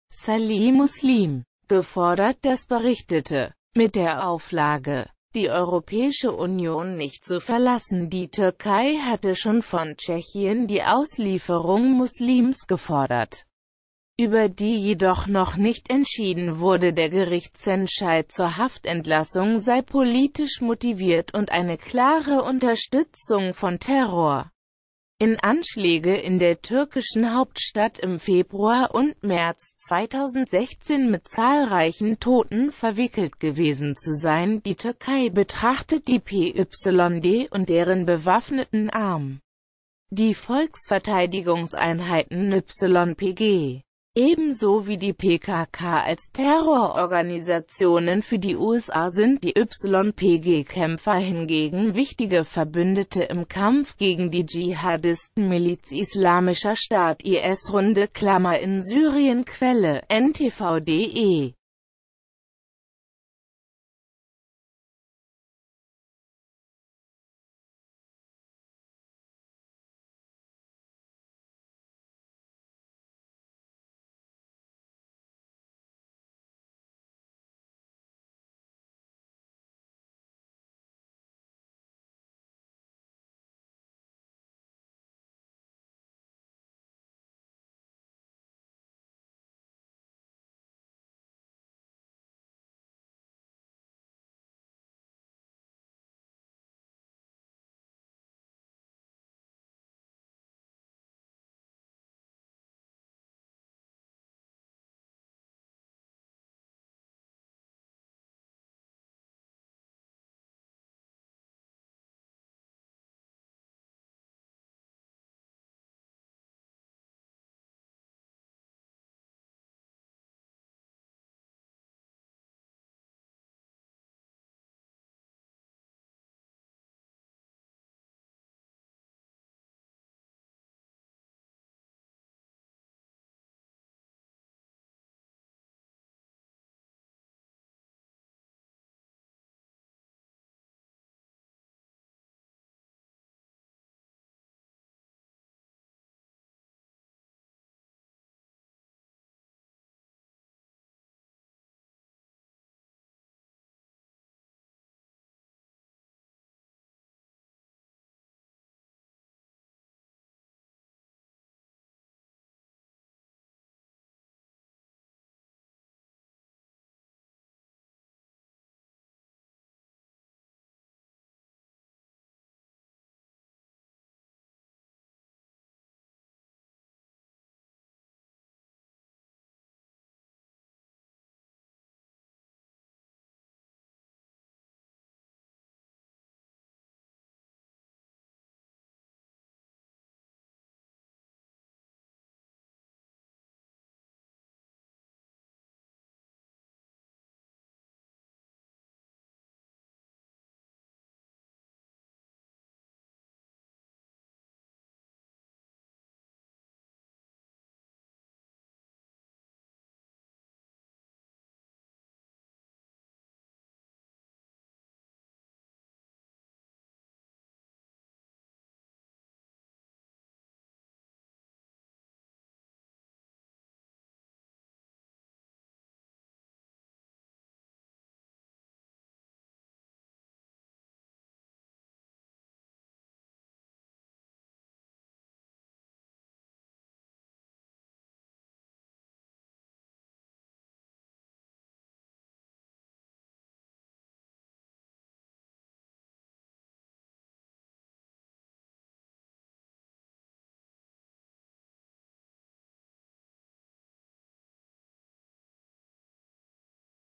Bundesregierung soll ausliefern: Türkei verlangt Festnahme von Ex-PYD-Chef
Thanks you verry much, like and sub video channel:► car shop help me translate
。Die türkische Regierung macht den früheren Chef der kurdischen Partei PYD mitverantwortlich für Terroranschläge in der Türkei Der Politiker hält sich zurzeit in der EU auf Nach Tschechien soll ihn nun Deutschland festnehmen und ausliefern。Die Türkei hat von Deutschland die vorübergehende Festnahme und Auslieferung des ehemaligen Chefs der syrischen Kurdenpartei PYD, Salih Muslim, gefordert Das berichtete, die staatliche Nachrichtenagentur Anadolu Der von der Türkei gesuchte Muslim hatte am Wochenende in Berlin an einem Protest gegen die türkische Militäroffensive im Nordwesten Syriens teilgenommen Der Einsatz richtet sich gegen die Kurdenmiliz YPG, die eng mit der PYD verbunden ist。Der 67 Jahre alte Muslim war Ende Februar aufgrund e